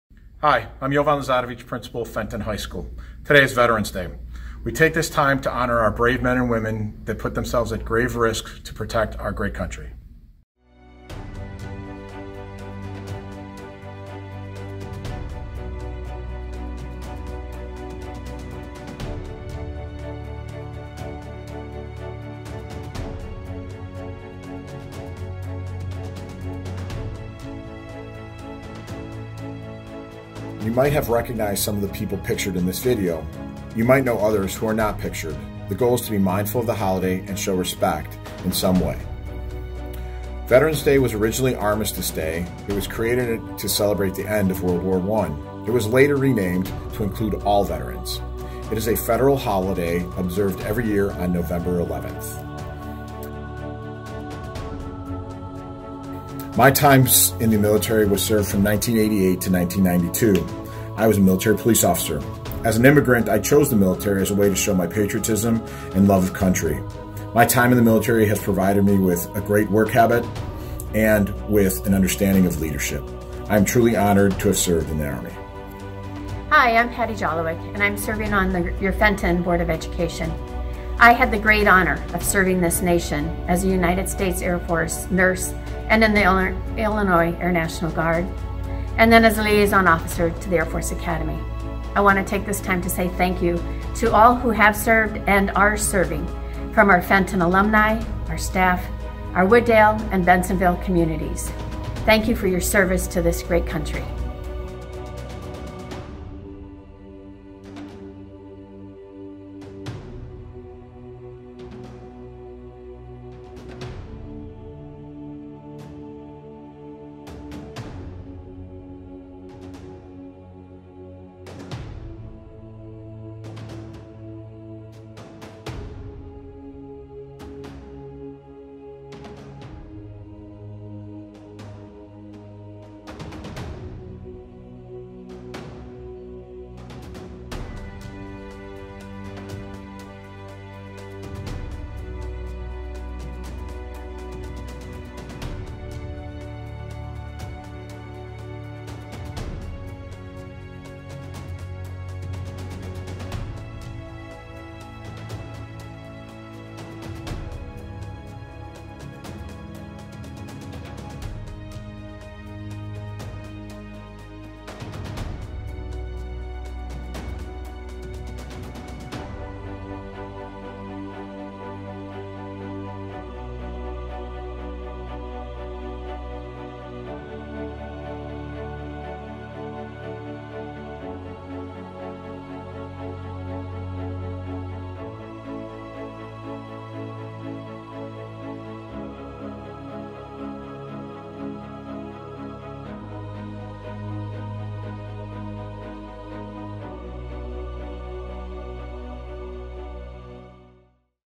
Fenton's 2020 Tribute to Veterans
Fenton High School Principal Jovan Lazarevic leads a Nov. 11, 2020 tribute to veterans featuring photos of staff members, recent graduates, family, and community members who served in the armed forces.